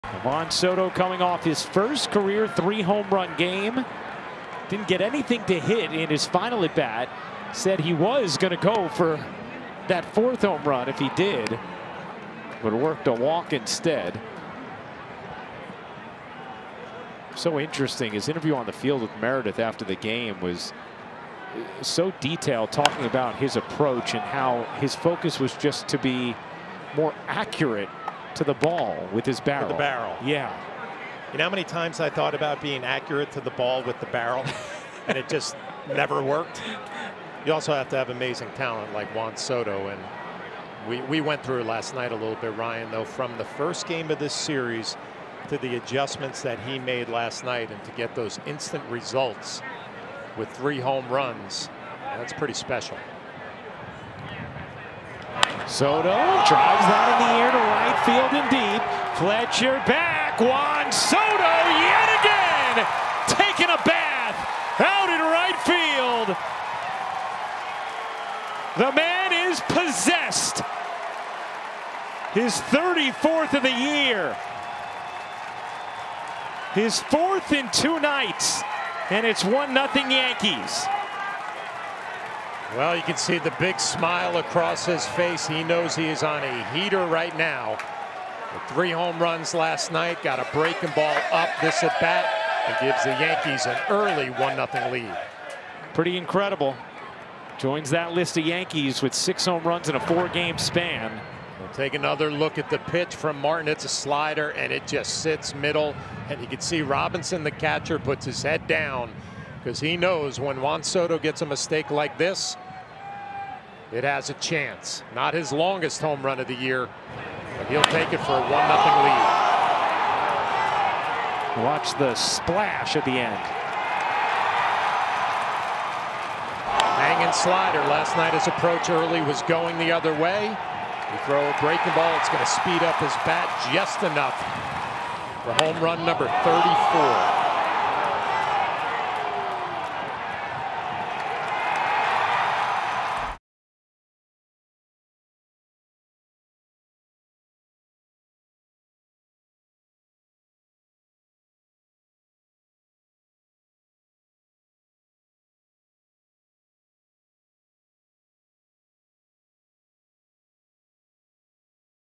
JUAN SOTO MAKES A SPLASH! (4th straight at-bat with a homer for the Yankees slugger!)
Juan Soto is scorching hot. The New York Yankees slugger CRUSHES this homer vs. the Chicago White Sox.